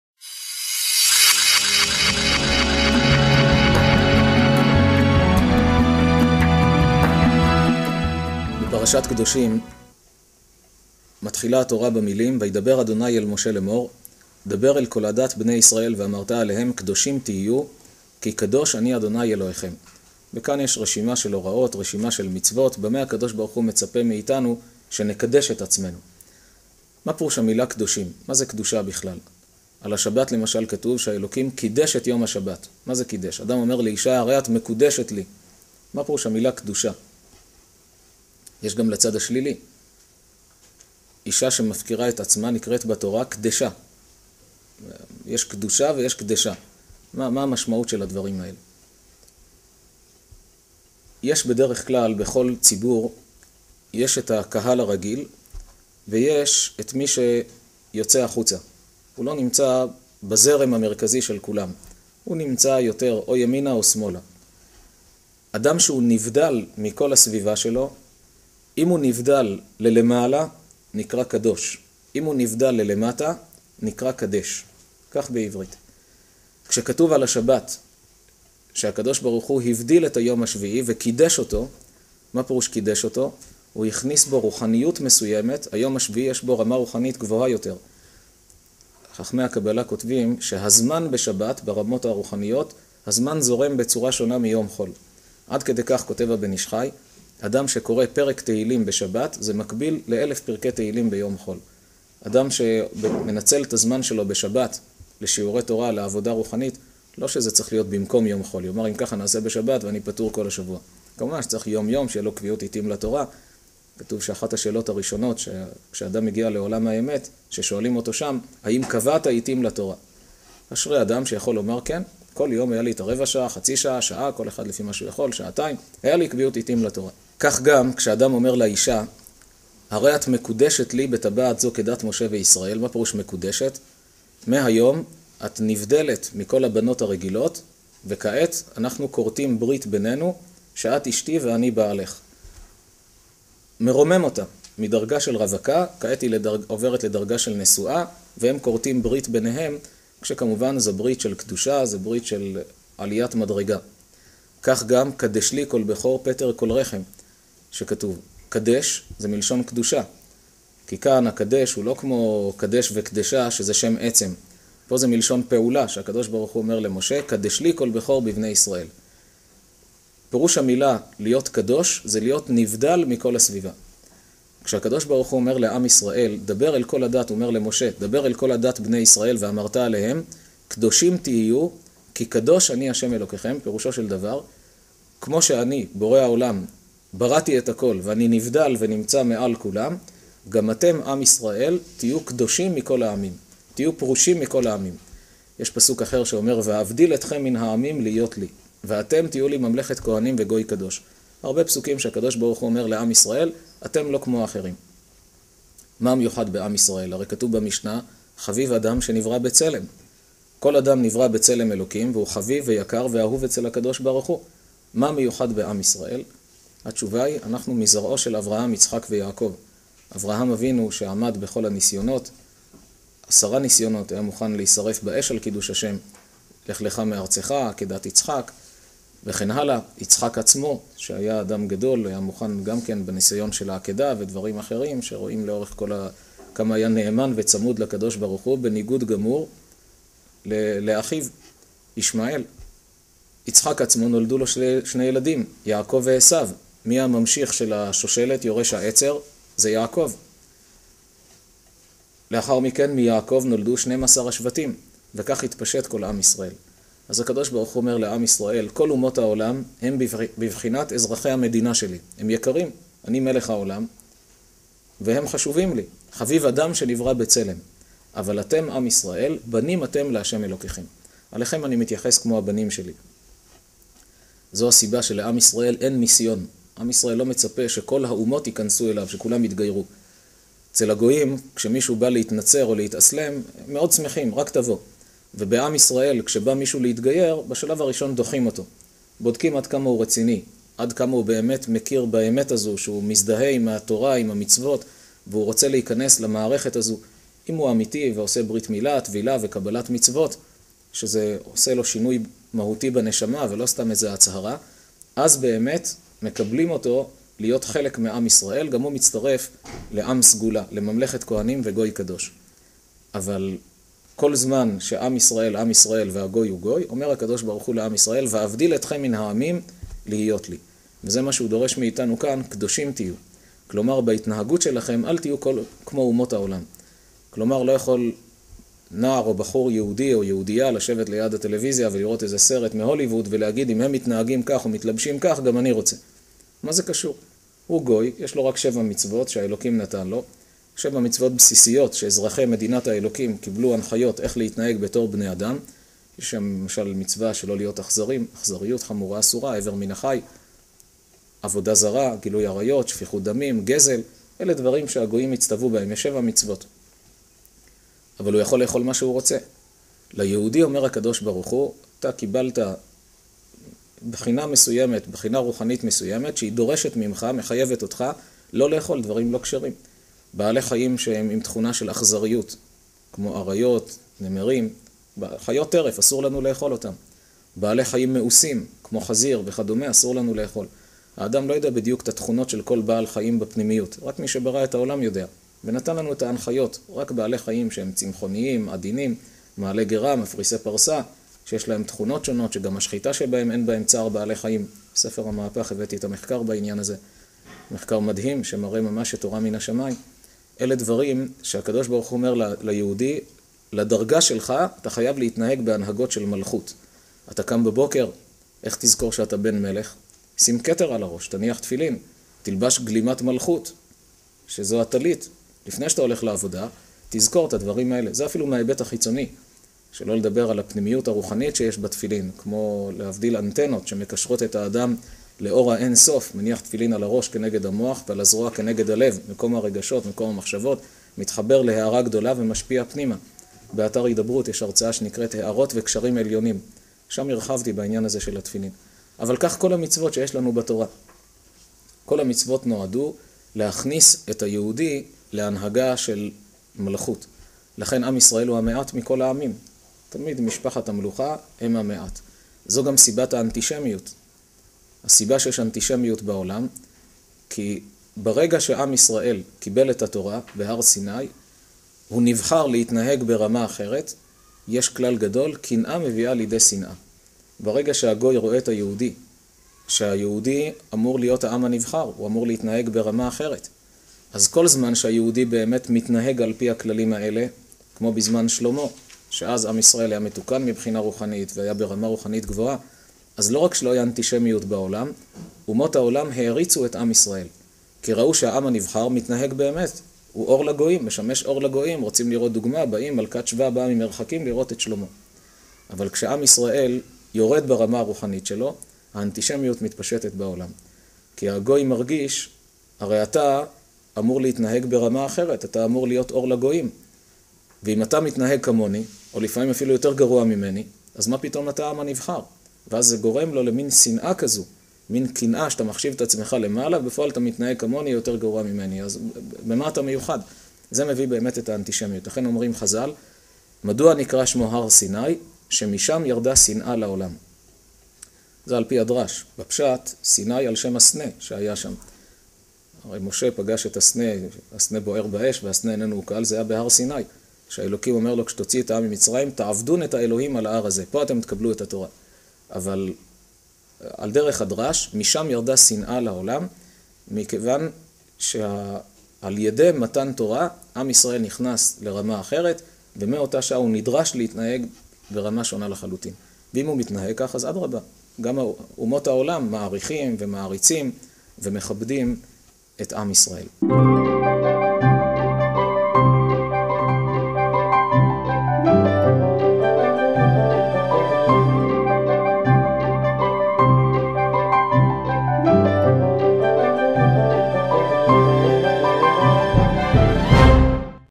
פרשת קדושים - קדושת ישראל
פרשת פרשת השבוע עם הרב זמיר כהן